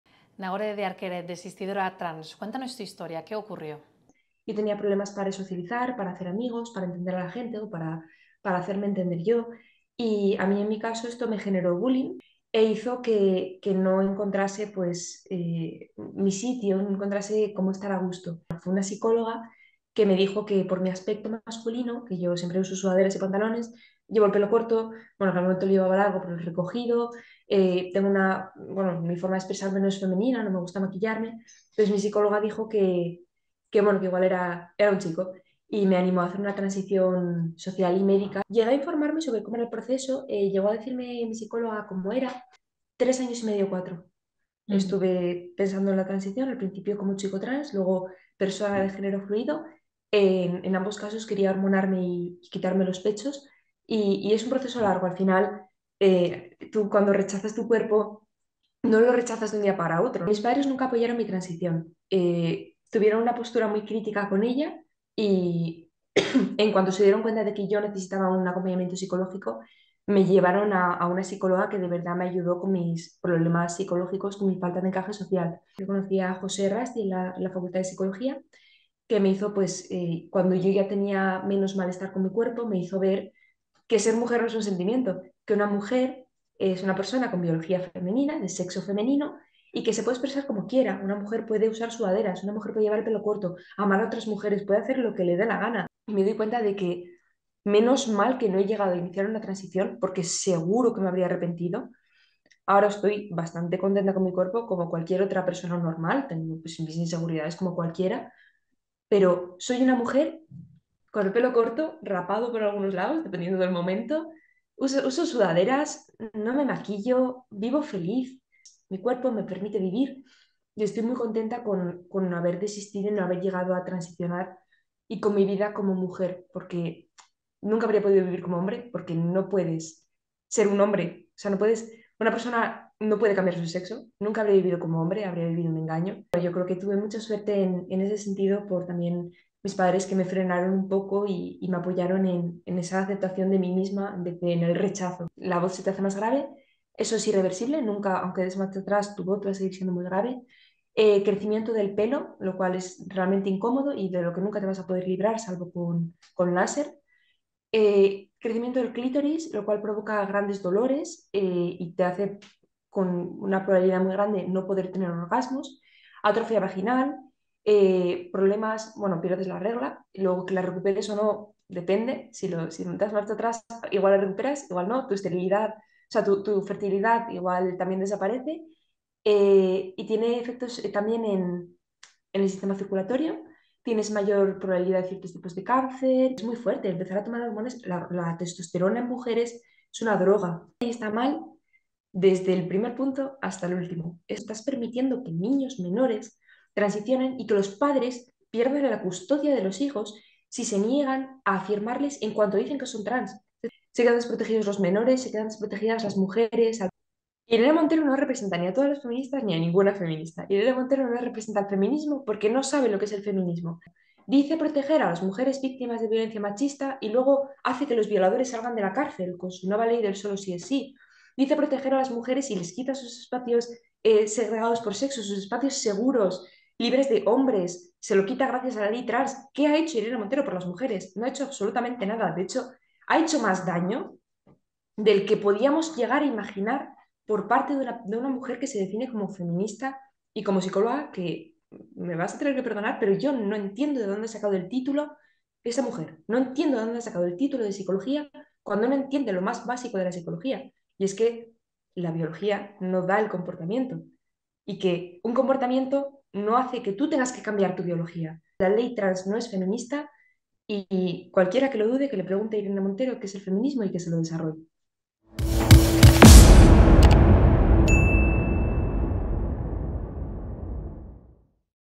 Nagore, la mujer que desistió ser trans: «Una psicóloga me animó a ser chico por aspecto masculino»
La ley trans de Irene Montero ya es ley y permite a los menores de edad cambiar su sexo registral sin el consentimiento de sus representantes legales. Nagore de Arquer se planteó esta posibilidad cuando sólo era una adolescente, sin embargo, dio marcha atrás. Nagore estuvo a punto de hormonarse y quitarse el pecho, pero algo le hizo cambiar. El apoyo de sus padres, junto con ayuda psicológica, motivó que la adolescente diera marcha atrás y desistiera del proceso de transición entre sexos que iba a comenzar. «Una psicóloga me animó a ser chico por mi aspecto masculino», relata en una entrevista con OKDIARIO en la que cuenta su historia y su rechazo hacia la nueva norma impulsada por el Ministerio de Igualdad.